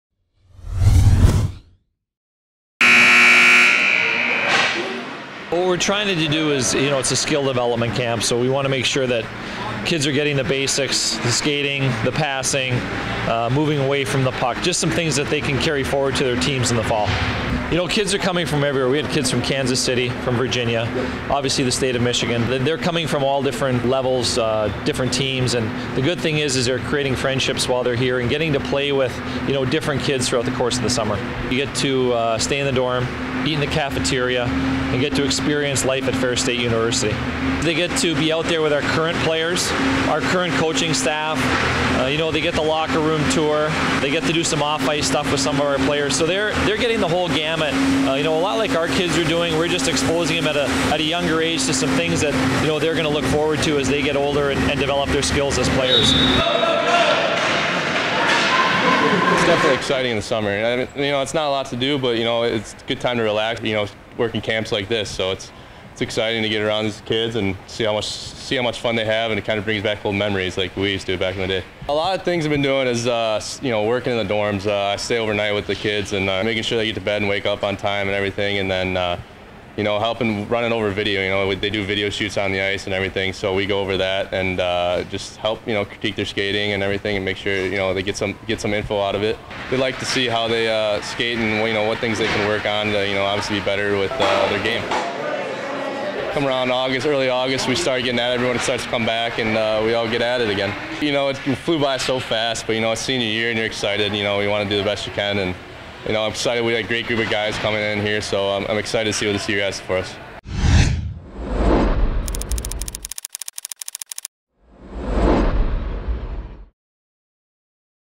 FSU Hockey Camp Summer 2016
Learn more about what goes on at Ferris State hockey camp in the summer. Hear from associate head coach Drew Famulak and senior defenseman Ryan Lowney.